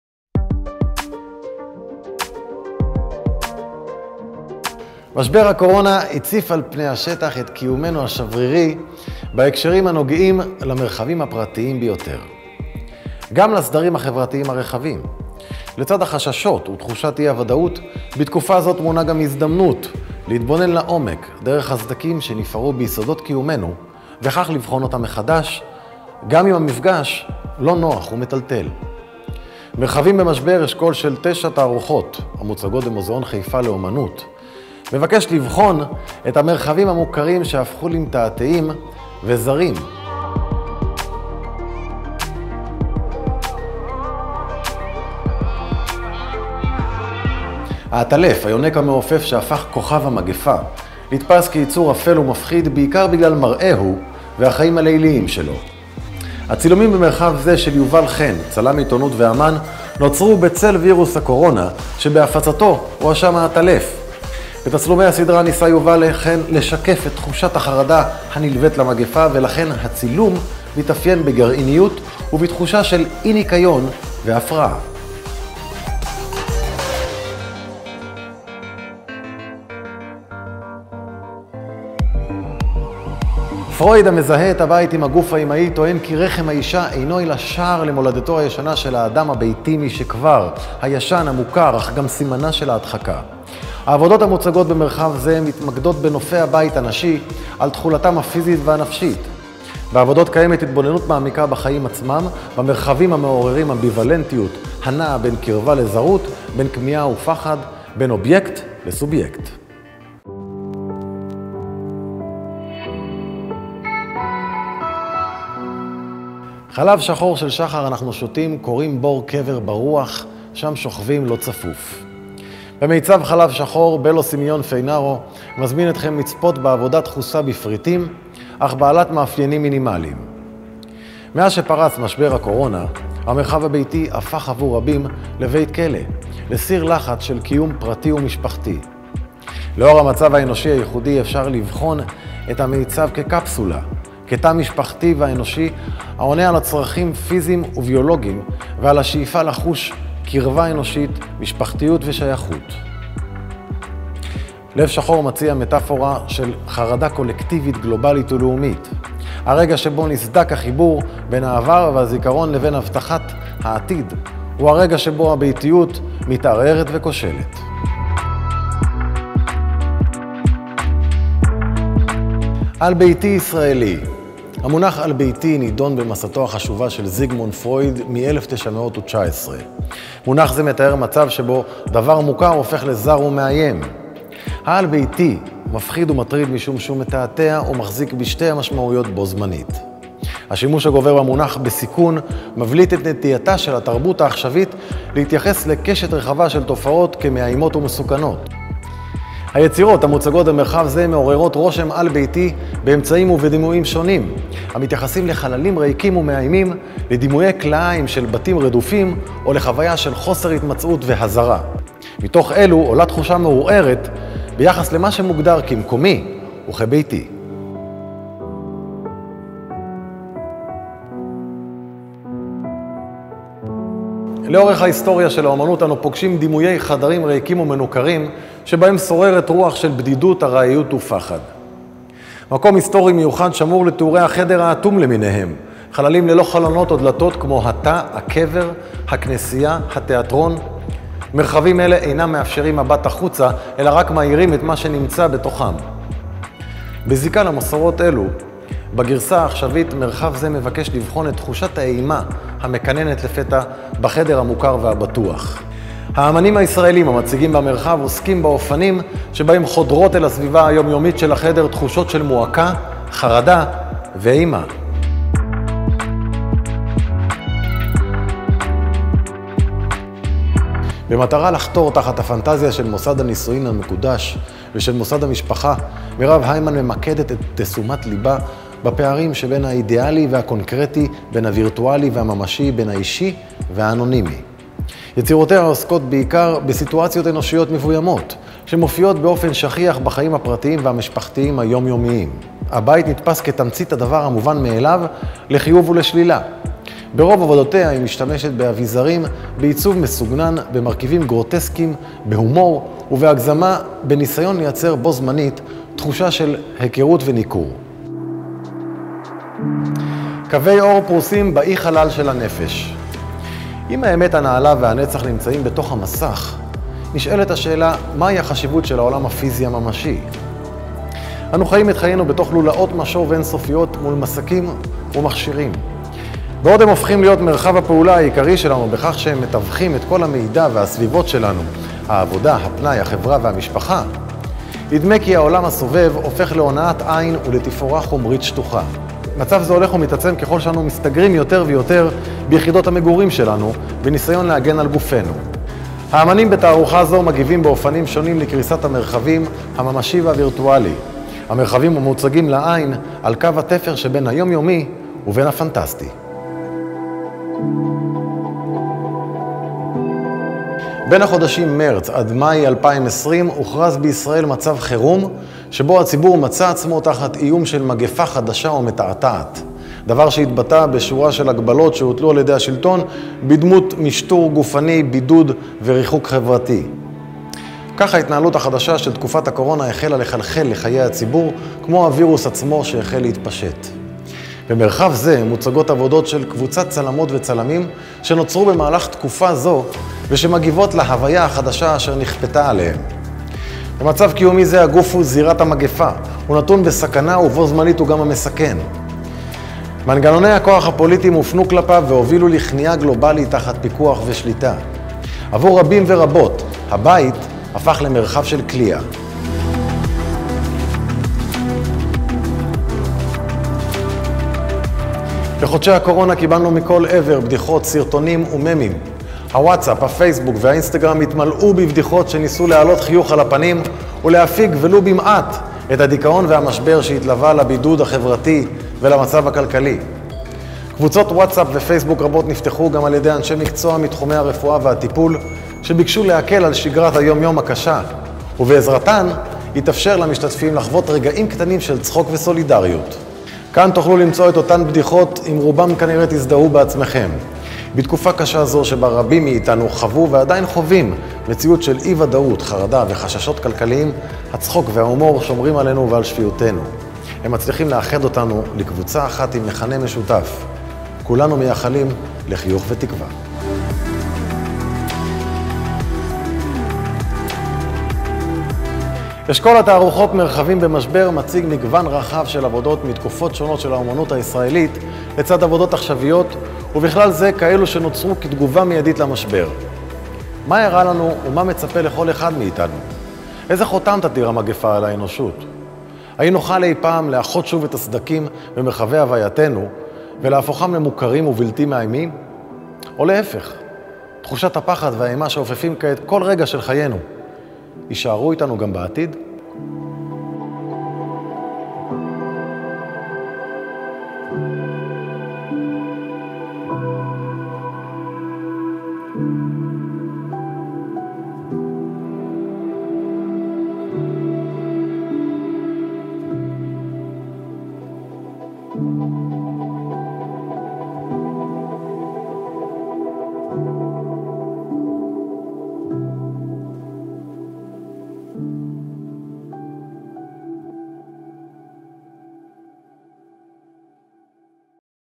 מרחבים במשבר | סיור וירטואלי במרחבי אשכול התערוכות עם רון שחר
משבר הקורונה, על השלכותיו המרובות, הציף על פני השטח את קיומנו השברירי בהקשרים הנוגעים למרחבים הפרטיים ביותר, כמו גם לסדרים החברתיים הרחבים. לצד החששות ותחושת אי־הוודאות, בתקופה זו טמונה גם הזדמנות להתבונן לעומק דרך הסדקים שנפערו ביסודות קיומנו וכך לבחון אותם מחדש, גם אם המפגש לא נוח ומטלטל.

סיור מיוחד שהופק במסגרת אשכול התערוכות "מרחבים במשבר" עם המנחה רון שחר.
במהלך הסיור רון שחר עובר דרך מרחבי התערוכה השונים ומעביר לנו מידע והסברים על היצירות ומקורות ההשראה של האמנים והאמניות ובכלל איך הכל מתקשר ומתחבר למרחבים הפרטים והאינטימיים שלנו שנמצאים כעת במצב של משבר.
הפקת וידאו:  Okopus // oktopus.co.il